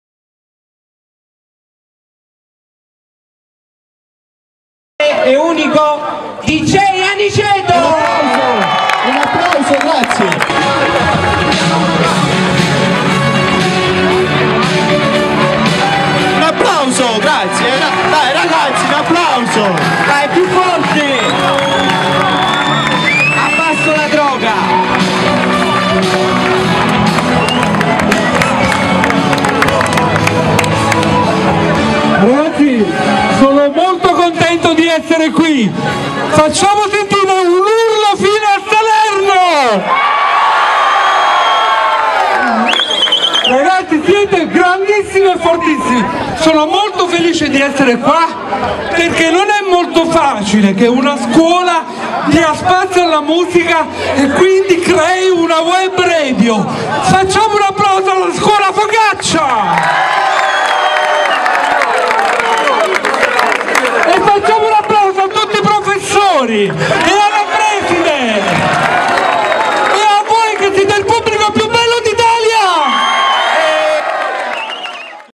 dj aniceto guest istituto focaccia sa
dj aniceto ospite dell'istituto superiore basilio focaccia di salerno per l'inaugurazione ddella web radio scolastica e come presidente di giuria del contest fra dj's della scuola.